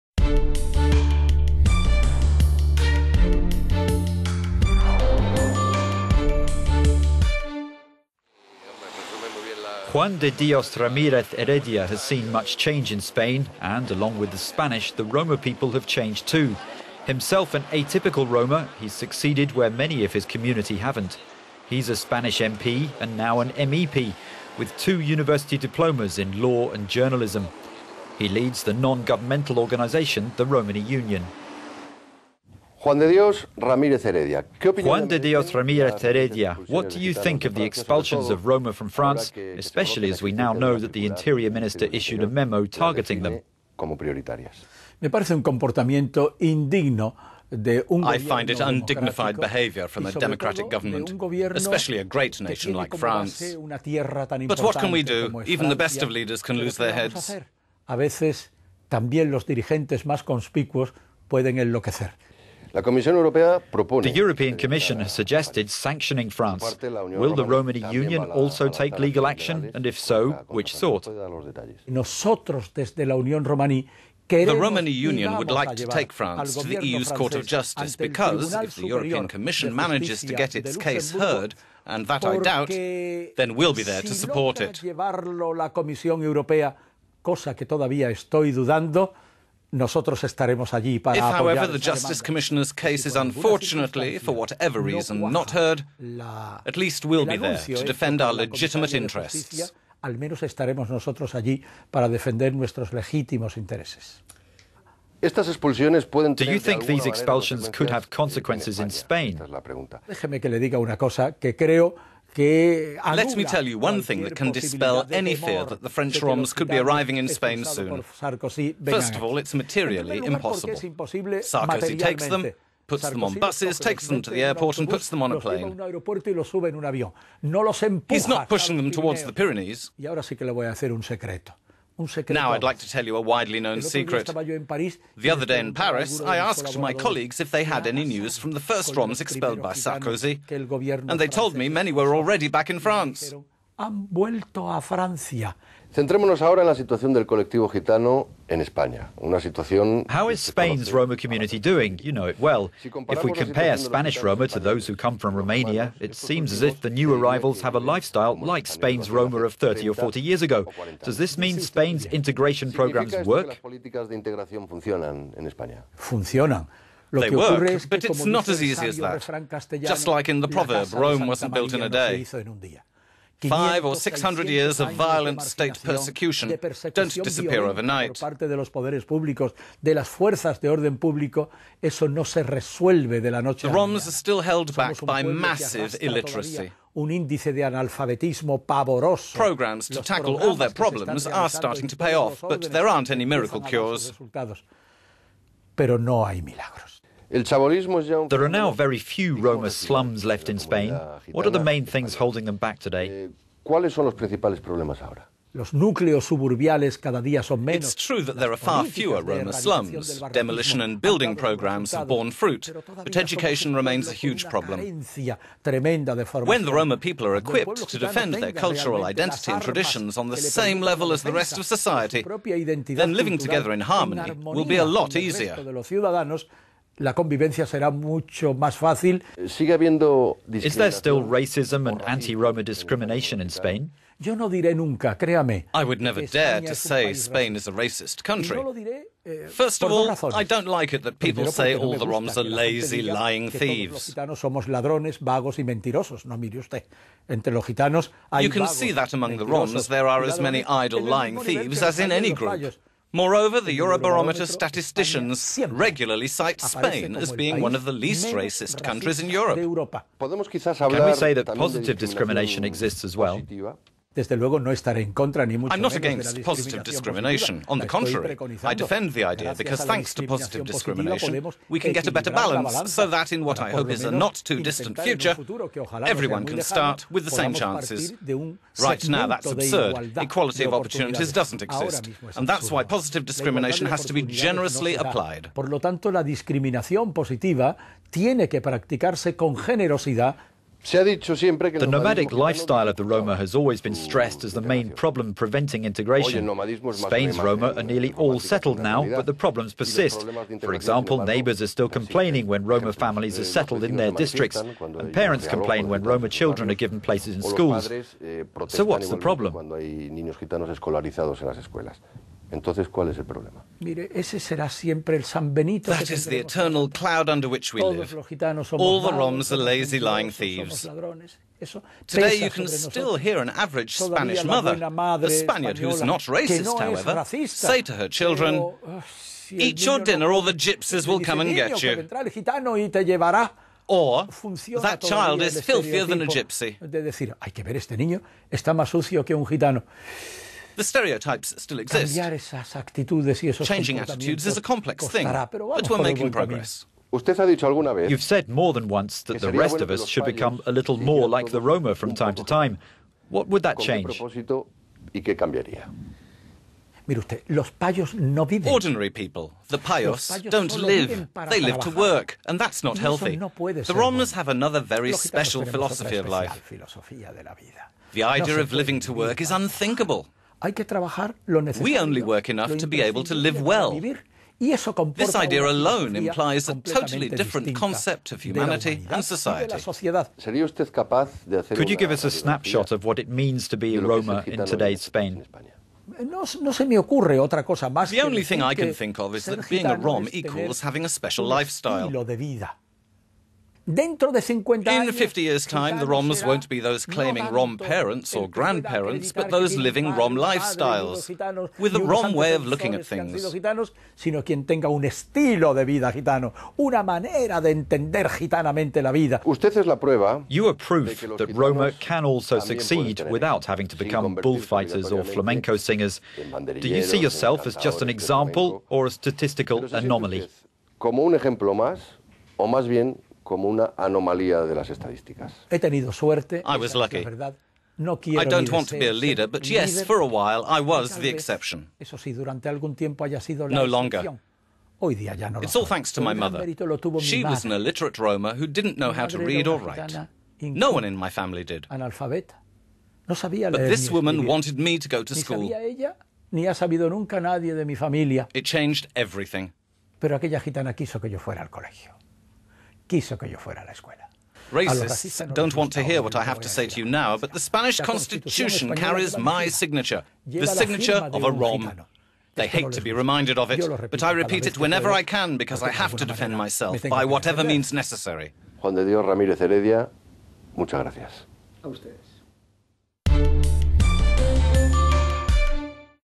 The Rom-arkable Juan de Dios Ramirez Heredia - interview
Juan de Dios Ramírez Heredia has seen much change in Spain, and along with the Spanish the Roma people have changed, too. Himself an atypical Roma, he has succeeded where many of his community has not. He is a Spanish MP, and now an MEP, with two university diplomas in law and journalism. In 1978 he helped write Spain's constitution and in 2008 he became the first-ever Roma holder of the title Doctor Honoris Causa from Cadiz university. He leads the non-governmental organisation the Romaní Union.